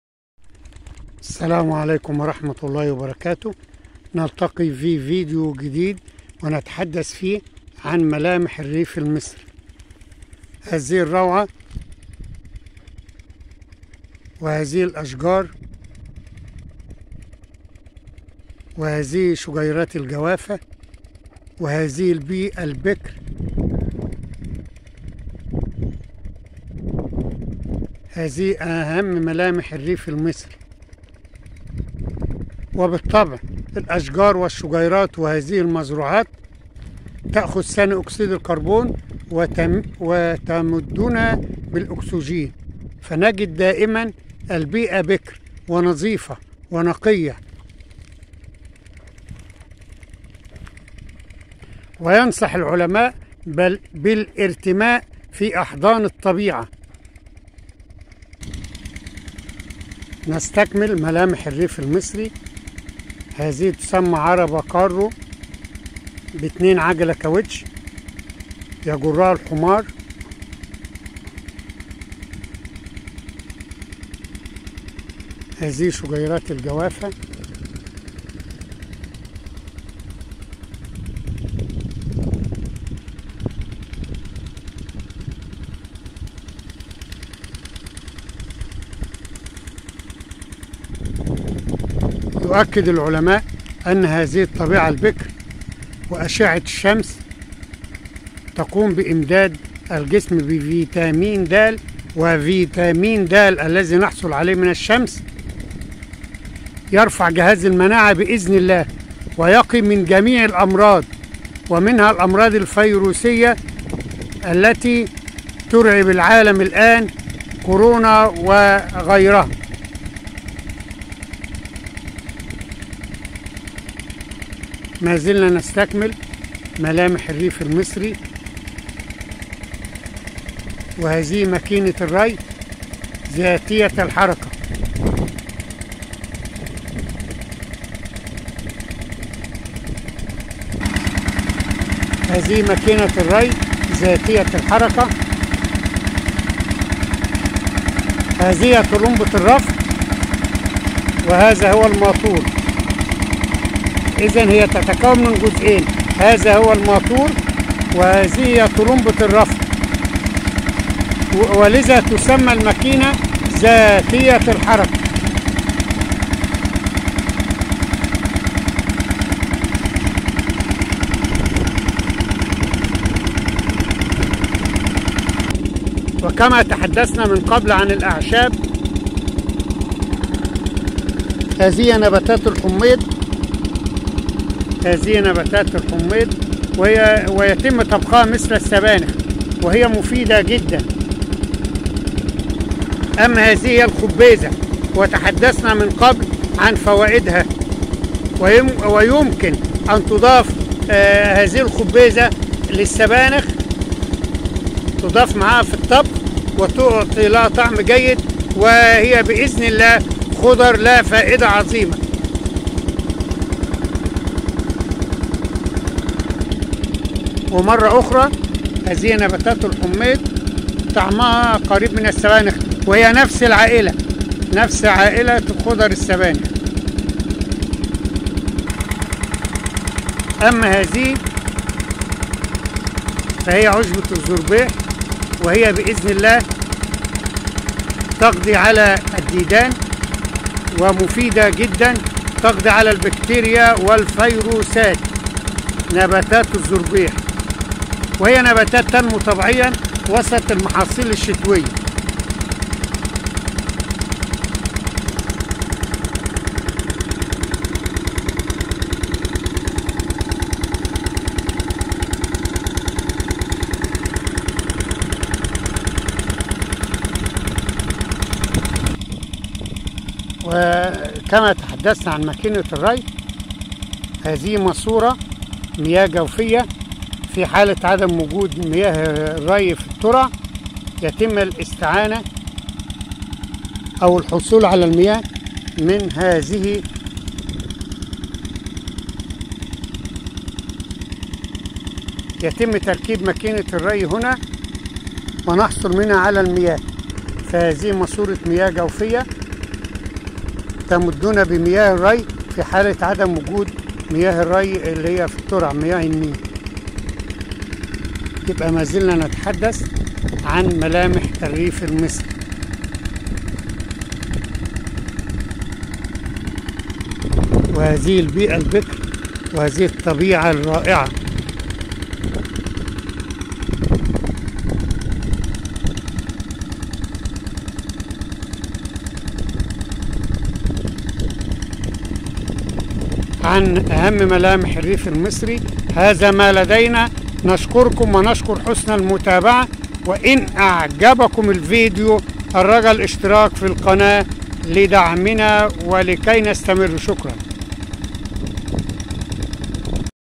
ماكينة الري،والعربة الكارو (ملامح الريف المنتج )
البقلية ،شاهد بعض ملامح الريف المنتج ، شاهد  ماكينة الري ،شاهد الحمار  والذي يقوم بنجر العربة الكترو، وينقل الاحمال من مكان لاخر